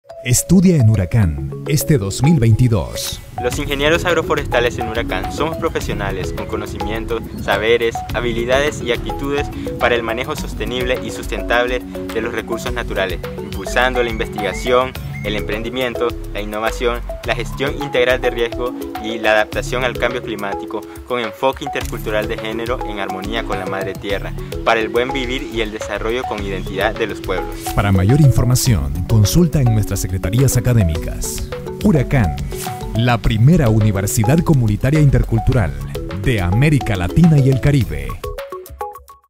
INGENIERÍAS: Ingeniería Agroforestal
Estudia en URACCAN, este próximo año 2022, conocé nuestra oferta académica

Ingeniería Agroforestal:
• Bilwi, Bluefields, Siuna, Rosita, Bonanza y Nueva Guinea

📢 Aprovecha la oportunidad de estudiar en “La Primera Universidad Comunitaria Intercultural en América Latina y el Caribe”.

📑 Pre-matrículas: octubre 2021 al 10 enero 2022
🤓 Propedéutico: 5 al 28 enero al 2022 para regular